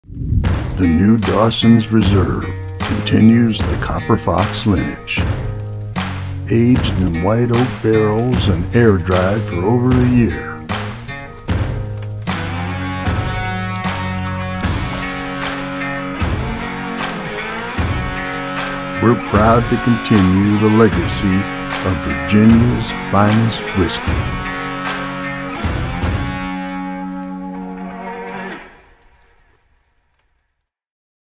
Pinnacle | CopperFox | Dawson's Reserve
Distilled in the heart of Williamsburg, Virginia, the Dawson's Reserve Bourbon  Whiskey continues the lineage of Virginia's finest whiskey.

This small batch uses hand malted 2 and/or 6 row Virginia barley medium smoked over 100% peachwood.

Aged in white oak barrels and air dried for over a year this mature, yet smooth batch is a proud continuation of the CopperFox traditions.